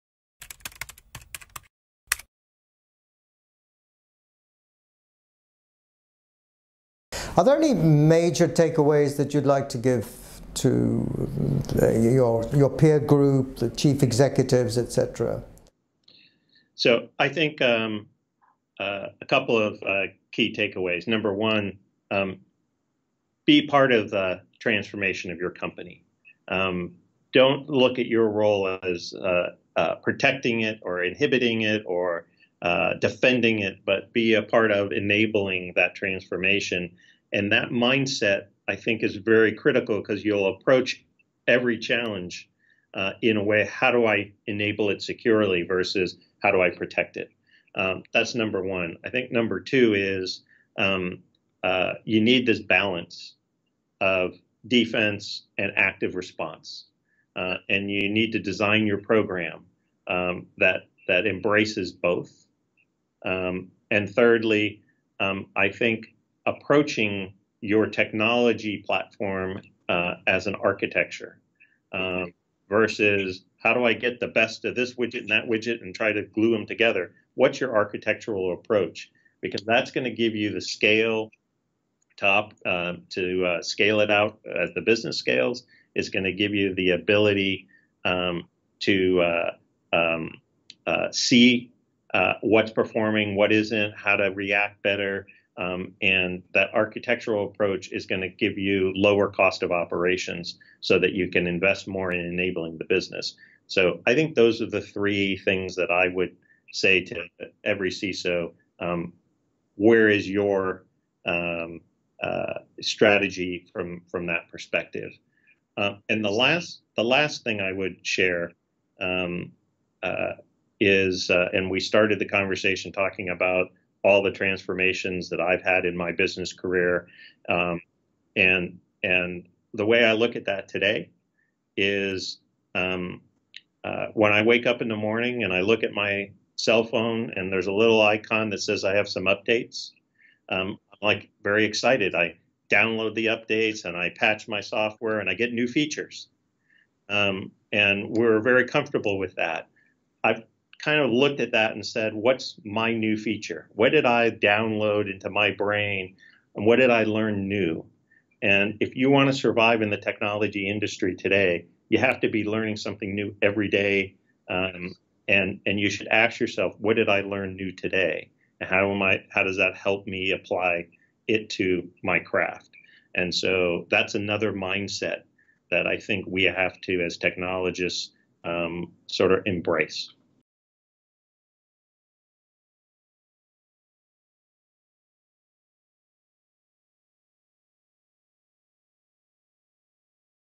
A message to security executives
Steve Martino has held senior leadership positions at numerous technology businesses over the course of his career. As Chief Information Security Officer at Cisco Systems, he has a message he would like to share with the security community on how to approach the space and what he believes is round the corner.